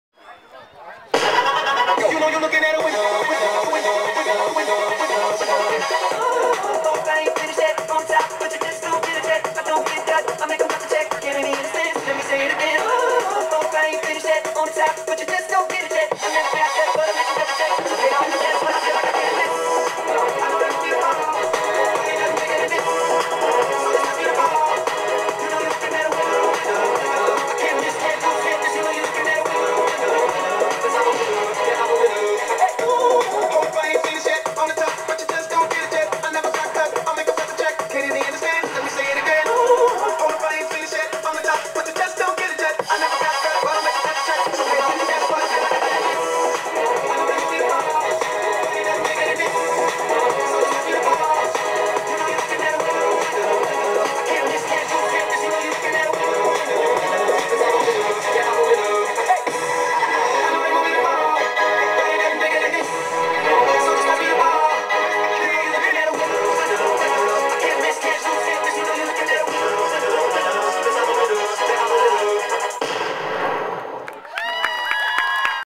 Linn High School cheer routine
The Linn (KS) High School cheer routine at halftime of the homecoming football game on Oct. 8, 2010.